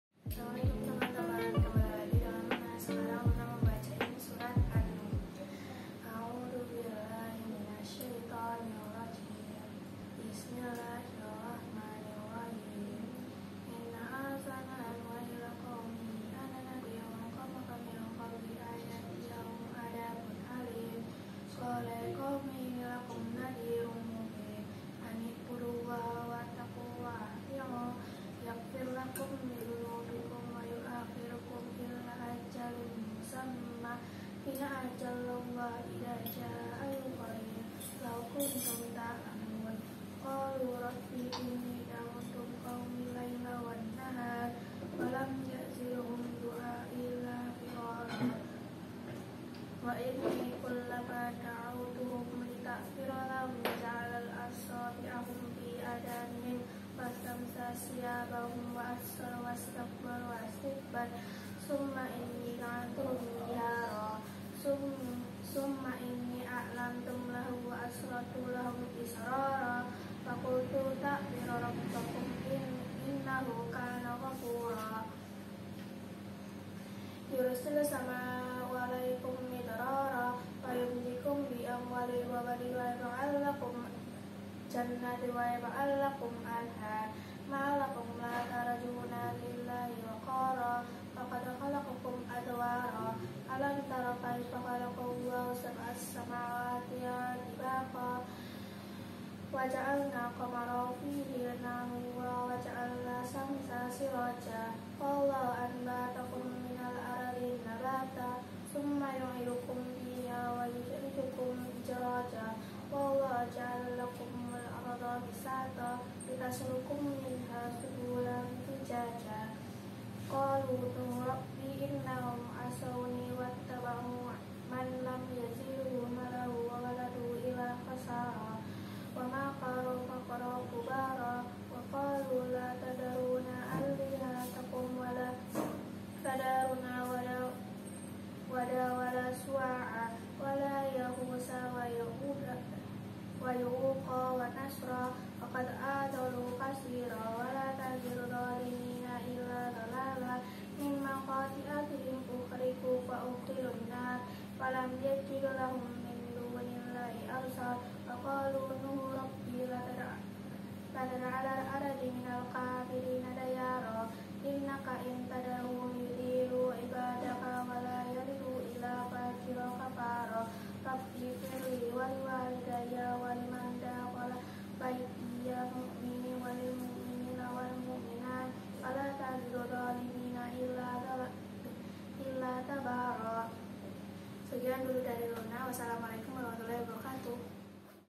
Pembacaan Qur'an surah An-Nuh [Ayesha Luna]
Hai Guys vidio kali ini Ayesha mau bacain Qur'an Surah An-Nuh

Semoga Dapat bermanfaat bagi teman teman di rumah yaa ^_^
Selamat menjalankan ibadah puasa bagi yang menjalankan

Insyaallah akan di banyakan konten seperti ini untuk kedepannya